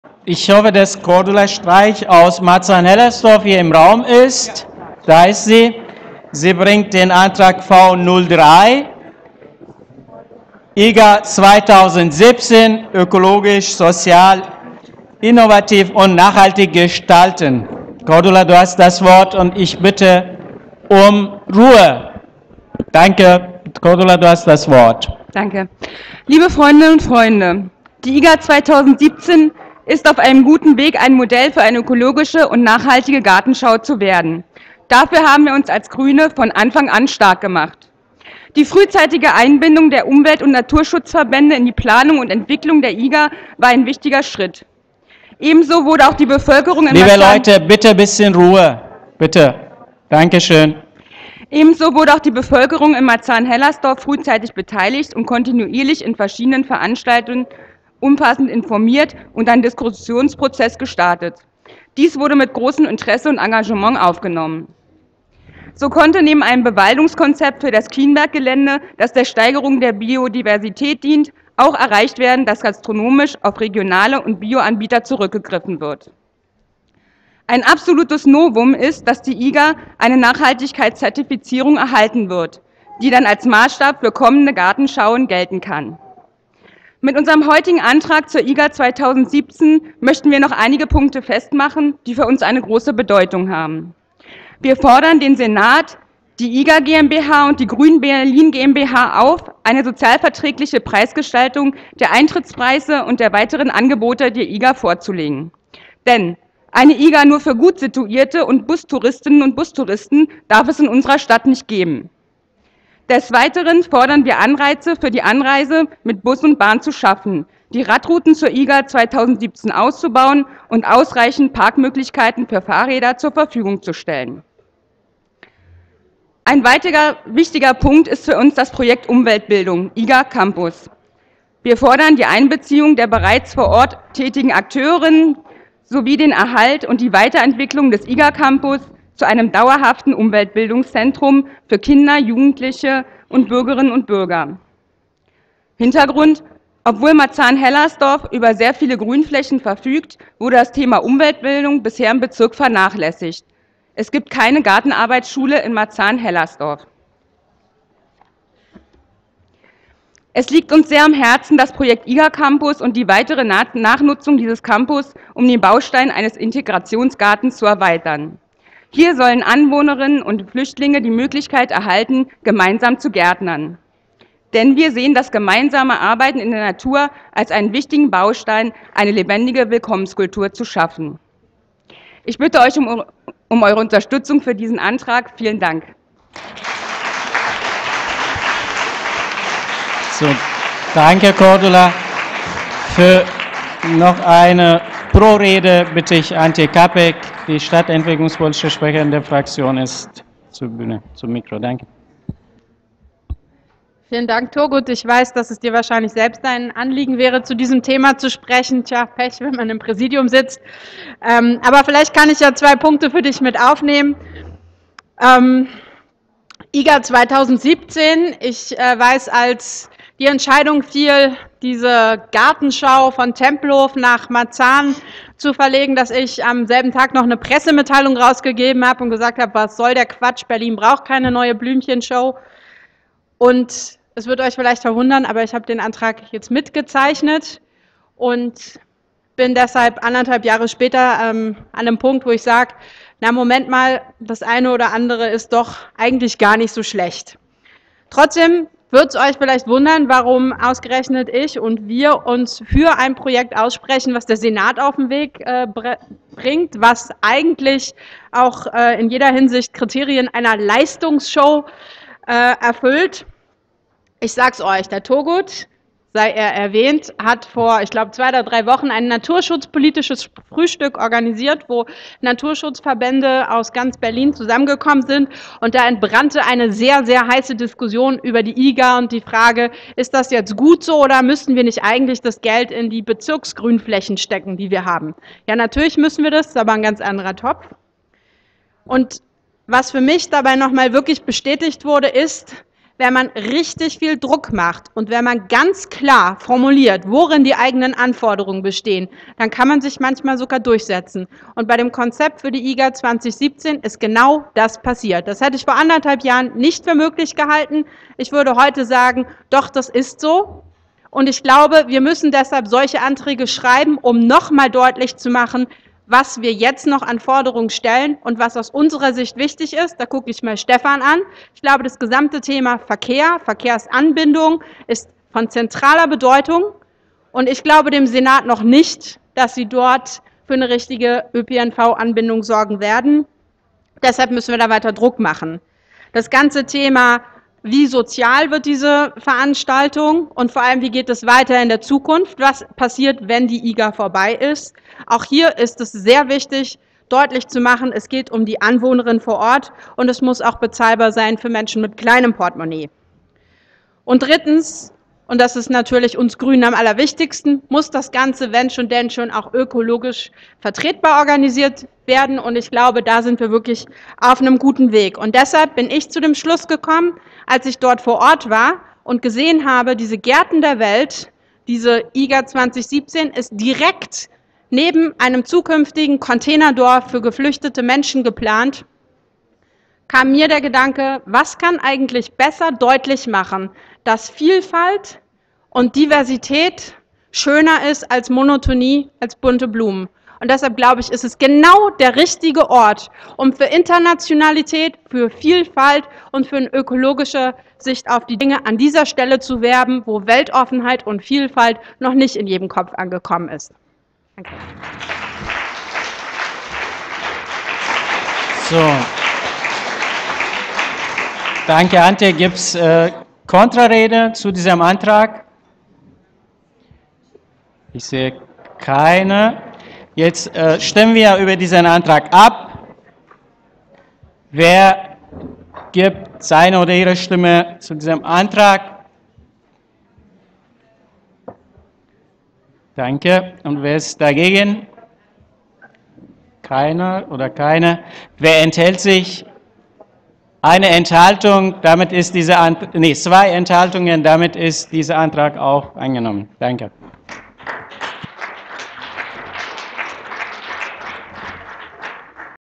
V-03: IGA 2017 ökologisch, sozial, innovativ und nachhaltig gestalten - TOP 10
V-03: IGA 2017 ökologisch, sozial, innovativ und nachhaltig gestalten -  TOP 10 - Parteitag Bündnis 90/Die Grünen Berlin